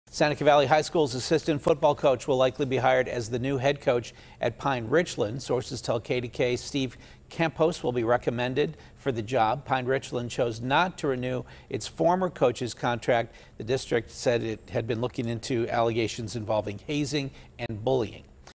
Sources: Seneca Valley Assistant Coach Could Be Hired As Pine-Richland's Head Football Coach
Seneca Valley's assistant football coach is likely to be hired as Pine-Richland's new head football coach, sources tell KDKA.